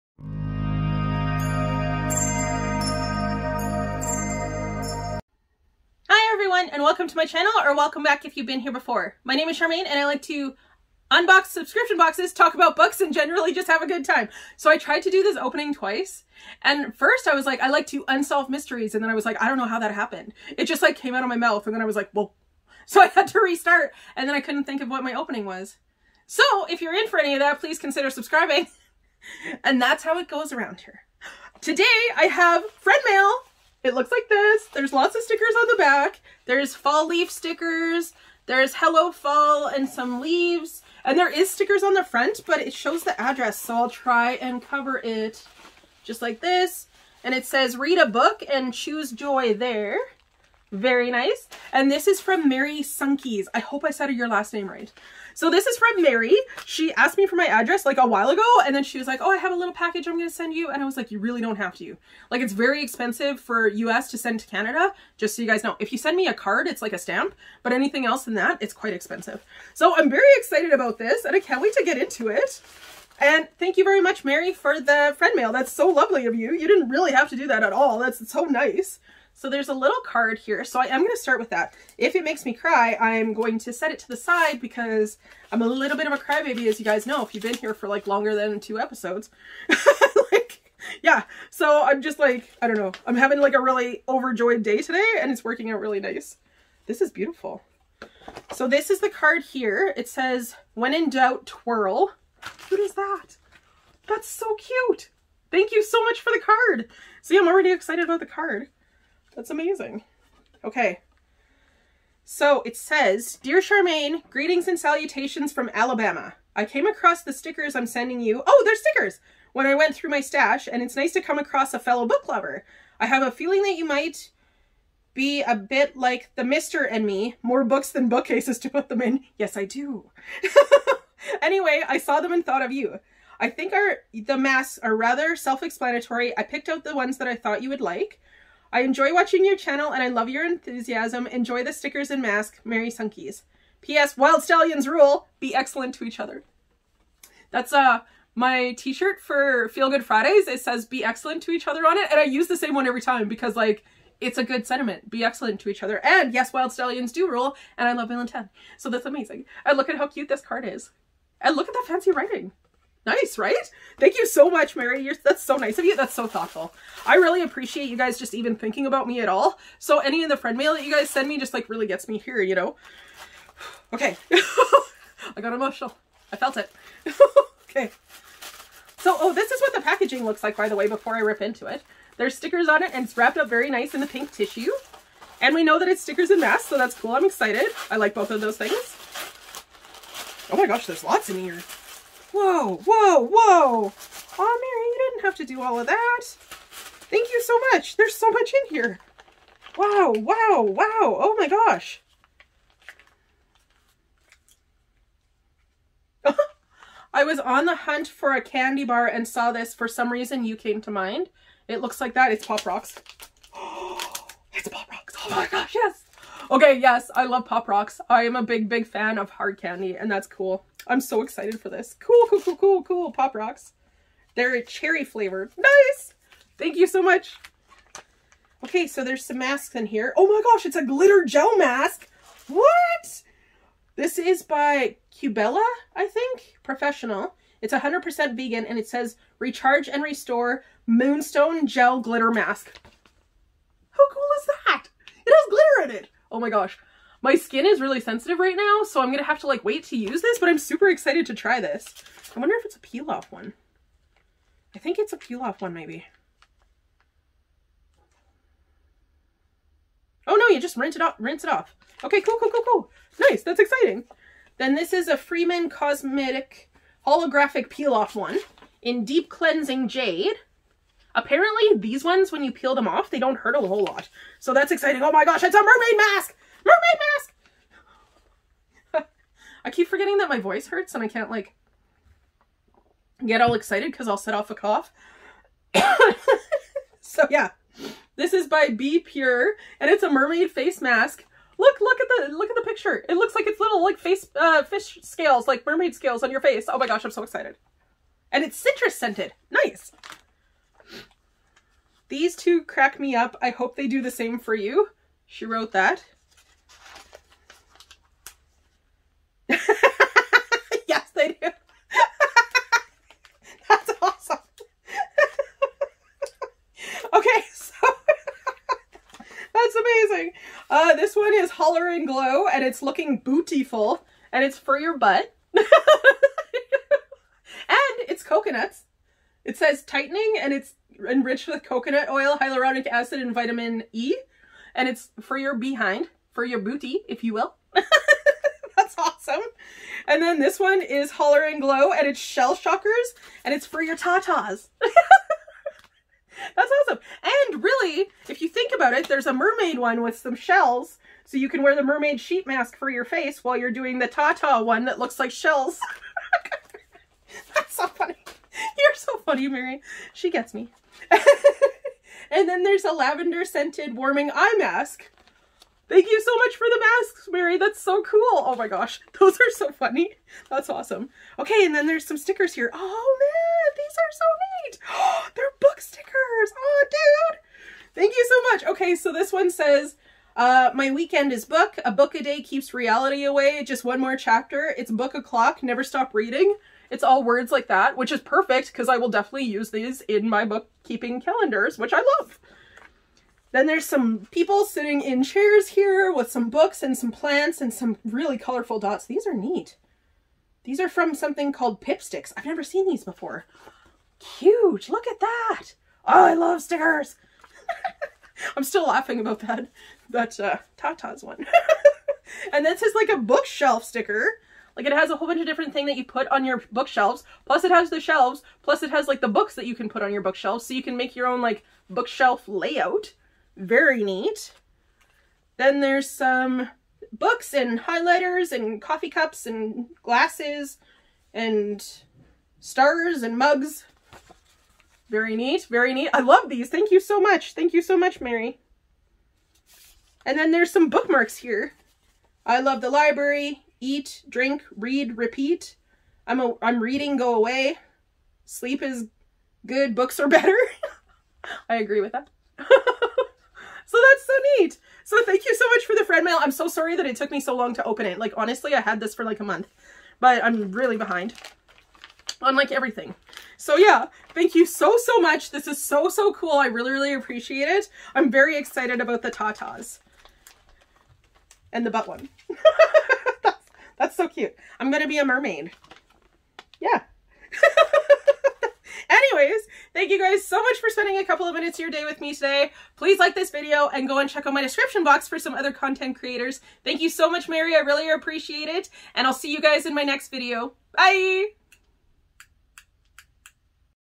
Mask Monday Episode #33! Friend Mail From Mary S! ❤️
My name is Sharmaine. I like to unbox subscription boxes, talk about books and have fun.

Thank you so much Mary Sunkes 🤗❤️🤗

The following links may or may not give me points:

🌟 Cozy Book Nook Subscription Box 💥 Let them know I invited you.

🌟 Dollar Tea Club:
REF4NRUDN8WG7

I appreciate you all so much ❤️